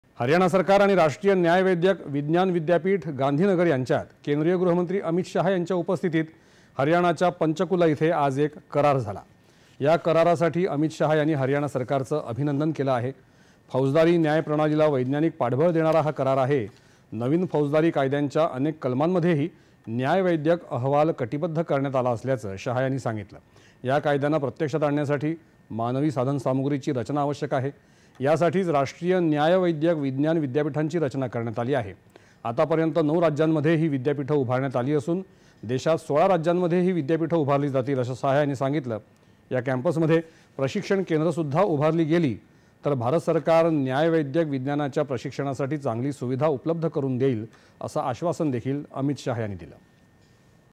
हरयाणा सरकार आणि राष्ट्रीय न्यायवैद्यक विज्ञान विद्यापीठ- गांधीनगर यांच्यात करार.
दि.२९.०६.२०२४ :
    हरयाणा सरकार आणि राष्ट्रीय न्यायवैद्यक विज्ञान विद्यापीठ- गांधीनगर यांच्यात, केंद्रीय गृहमंत्री अमित शहा यांच्या उपस्थितीत, हरयाणाच्या पंचकूला इथे आज एक करार झाला.  या करारासाठी अमित शहा यांनी हरयाणा सरकारचं अभिनंदन केलं. फौजदारी न्याय प्रणालीला वैज्ञानिक पाठबळ देणारा हा करार आहे,  नवीन फौजदारी कायद्यांच्या अनेक कलमांतही,  न्यायवैद्यक अहवाल कटिबद्ध करण्यात आला असल्याचं शाह यांनी सांगितलं.  या कायद्यांना प्रत्यक्षात आणण्यासाठी मानवी साधनसामुग्रीची रचना आवश्यक आहे, यासाठीच  राष्ट्रीय न्यायवैद्यक विज्ञान विद्यापीठांची रचना करण्यात आली आहे. आतापर्यंत ९ राज्यांत ही विद्यापीठं उभारण्यात आली, असून देशात सोळा राज्यात ही विद्यापीठं उभारली जातील, असं शाह यांनी सांगितलं. या कॅंम्पसमध्ये प्रशिक्षण केंद्र ही उभारलं गेल्यास, भारत सरकार न्यायवैद्यक विज्ञानाच्या प्रशिक्षणासाठी चांगली सुविधा उपलब्ध करून देईल, असं आश्वासन शाह यांनी दिलं.

8.30am | 11.00am | 1.00pm | 4.30pm | 7.00pm | 9.30pm